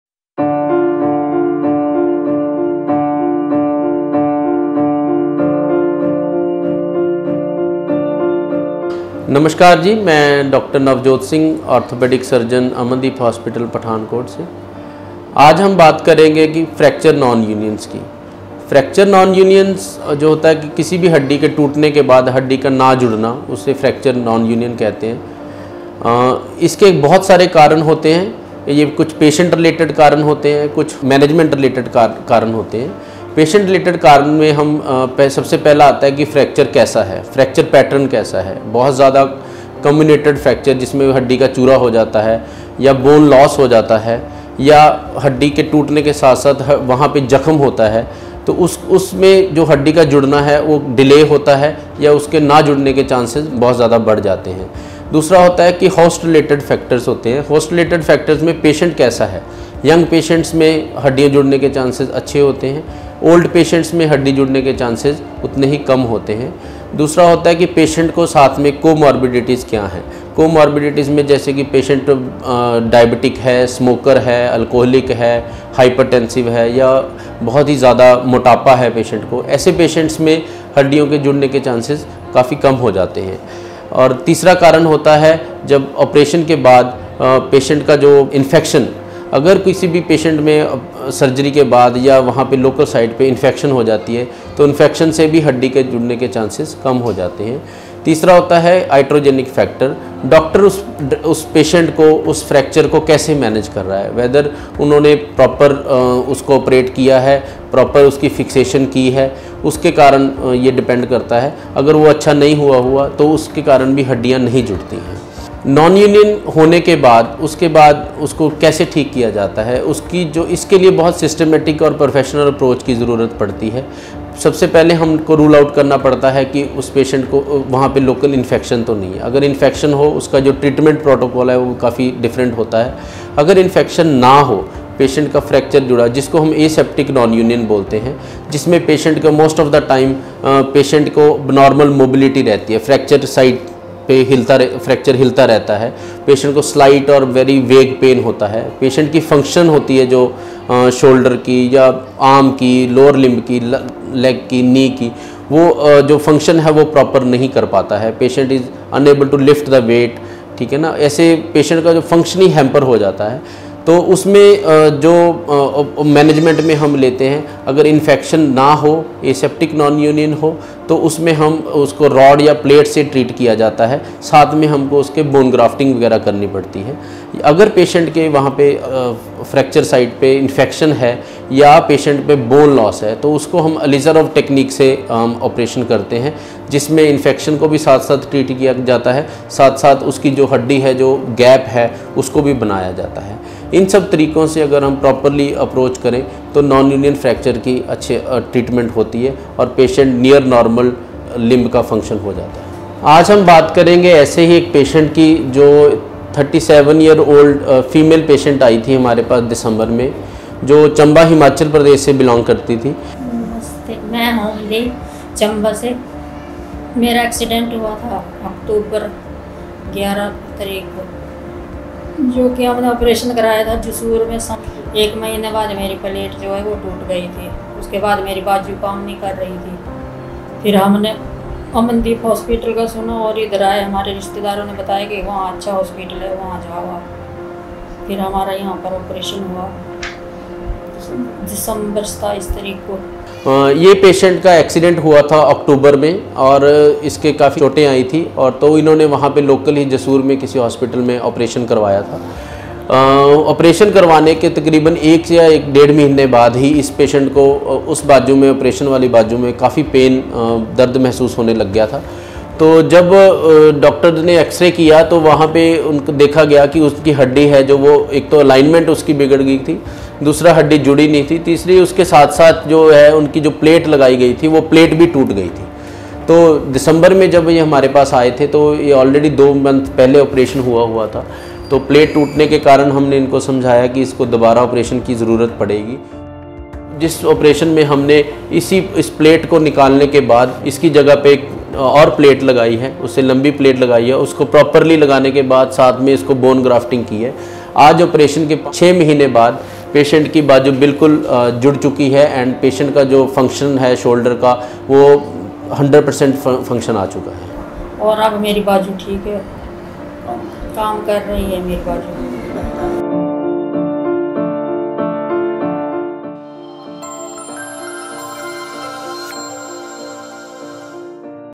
PATIENT TESTIMONIAL | NONUNION OF BONES | DR NAVJOT SINGH |AMANDEEP HOSPITAL
Welcome to this inspiring video where we will be sharing a patient's recovery journey from severe nonunion bones with the help of an expert orthopaedic doctor.

In this video, we are joined by a patient who shares their experience of being treated by Dr. Navjot Singh, a highly skilled orthopaedic surgeon at Amandeep Hospital. The patient had been suffering from severe nonunion bones and had tried various treatments without success. However, with Dr. Singh's expertise and personalized approach, the patient was able to recover and regain their mobility.

Through this video, the patient shares their experience of the treatment process and how Dr. Singh's approach helped them recover from their condition. The patient also provides valuable insights into the importance of finding the right doctor for your condition and how personalized treatment plans can make a significant difference in the recovery process.

We hope that this video serves as a source of inspiration and hope for those who may be struggling with nonunion bones or any other orthopaedic conditions. Dr. Singh's expertise and personalized approach have helped countless patients recover and regain their mobility, and we are excited to share one such inspiring story with you.

So, sit back, relax, and join us for this heartwarming session where a patient shares their journey of recovering from severe nonunion bones with the help of Dr. Navjot Singh from Amandeep Hospital.